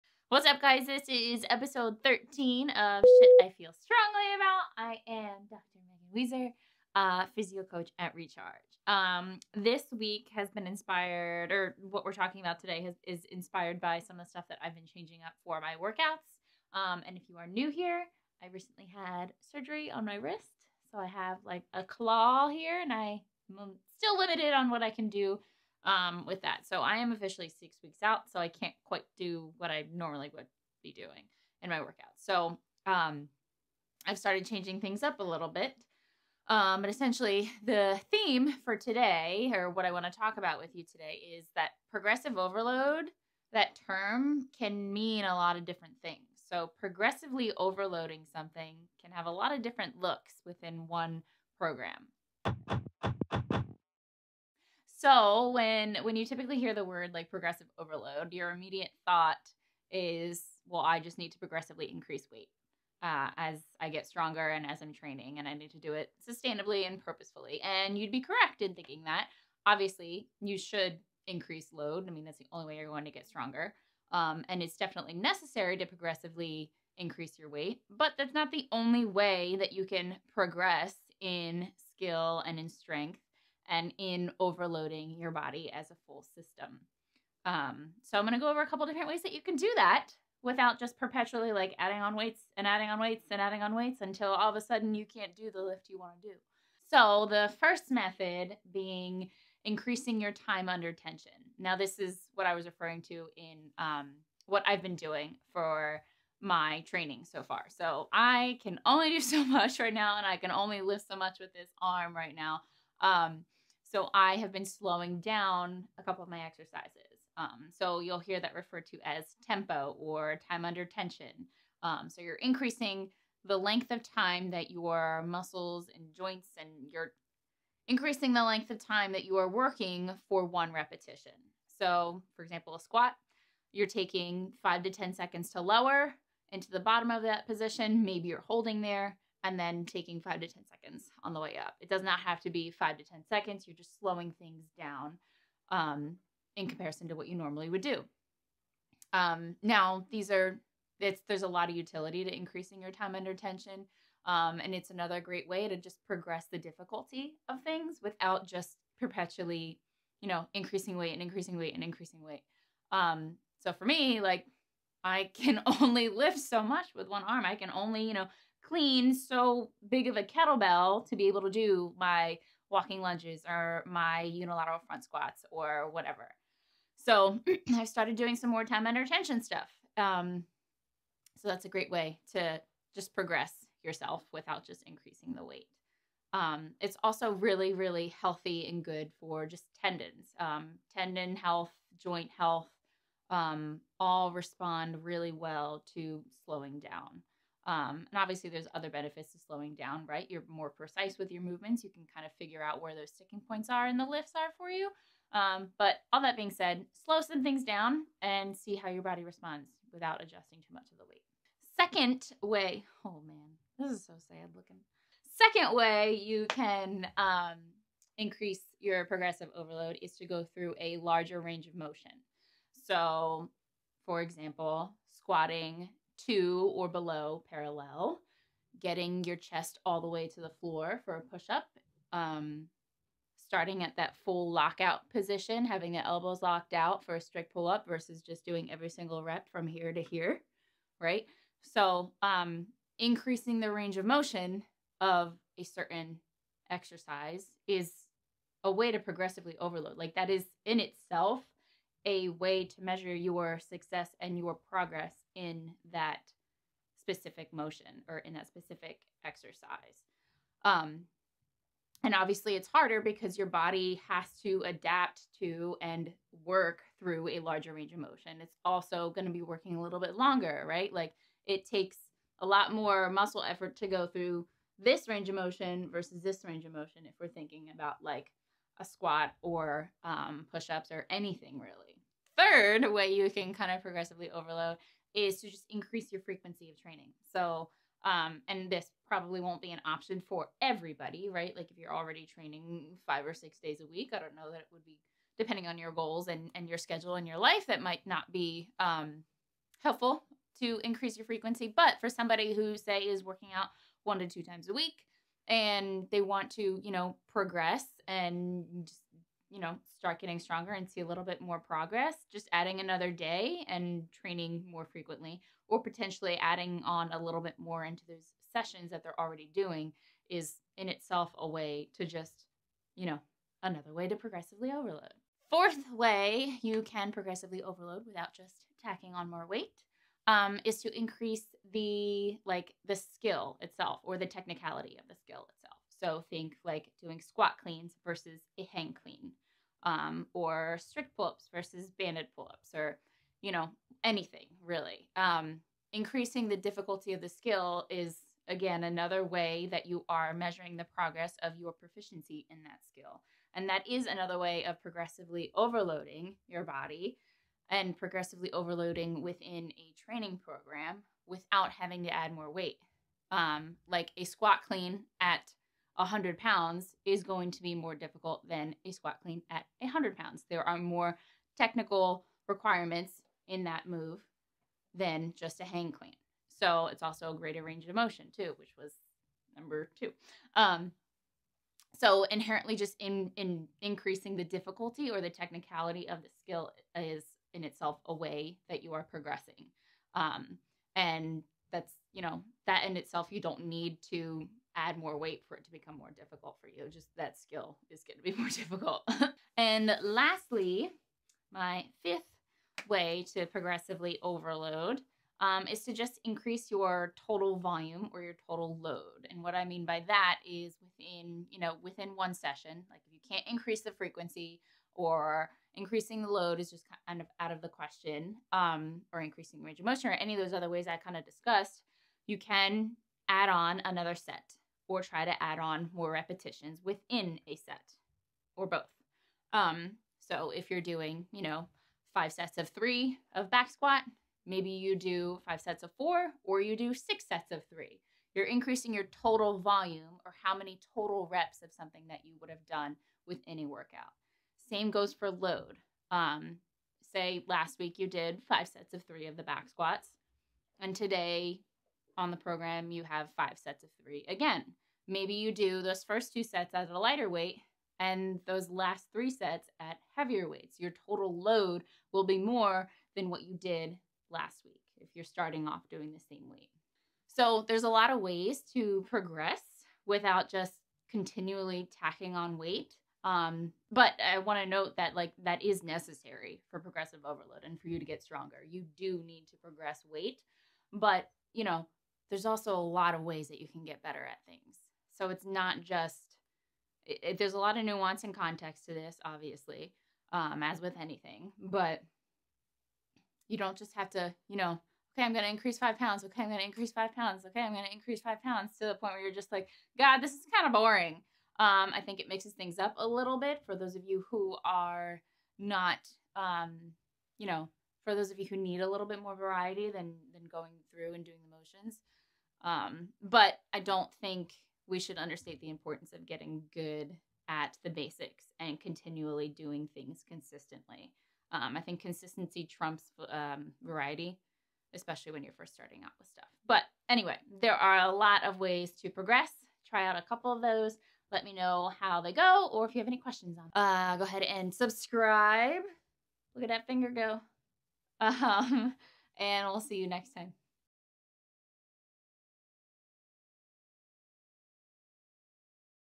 Train More Effectively with Various Progressive Overload Strategies
Have you ever heard the term “progressive overload” before?
Have you always thought that this meant progressively challenging your body and increasing the load you’re working with when you work out?
Well, you’d be correct…but you might not have the whole picture.
Progressive overload can mean different things, it doesn’t just have to mean increasing weight every single time you work out.
While that is necessary for improving strength, it’s not the only way to progress.
Watch this episode of SIFSA to find out more.

Train at home with Recharge: rechargemove.com